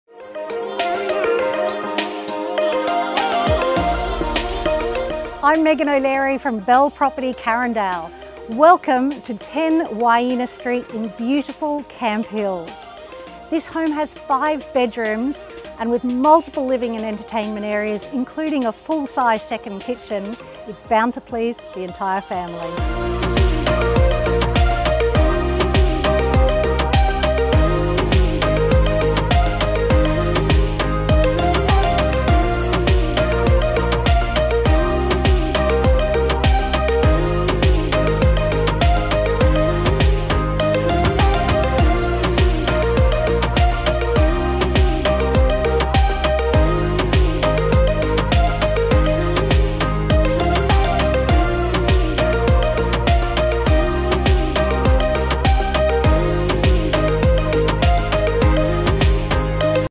10 Wyena Street, Camp Hill
City view living at its finest

This stunning two level, city view home is sure to please the whole family. With multiple living and entertaining areas including a full sized second kitchen, the home is perfectly suited to those requiring dual living, or those who simply have a love of poolside entertaining.

The architect designed renovation has made perfect use of the northly aspect to the rear of the home, incorporating all living and entertaining areas on this side.

Upstairs, the open plan living, dining, and kitchen area is the focus of the home. The kitchen has quality appliances, including induction cooktop and Miele dishwasher. The dark cabinetry beautifully offsets the rich hardwood timber floors throughout this area.  Family is the focus of the design, with an integrated study nook and perfect flow from living to rear deck. It’s here that you’ll never tire of the stunning city views whilst overlooking the pool and yard below.

The master retreat on this level has a walk in robe and private ensuite. Two further generous bedrooms on this level have built in robes, carpet, and ceiling fans. The family bathroom with separate toilet is centrally located.

Downstairs, a further two bedrooms, a combined living and dining and a full sized kitchen offer complete versatility. Perfect for older kids, extended family or for catering to guests or larger gatherings, the choice is yours.

Located a short distance from Perth St Park, direct City bus routes and several shopping and lifestyle options, this stunning home is certainly one to inspect. Call Megan O’Leary for more information.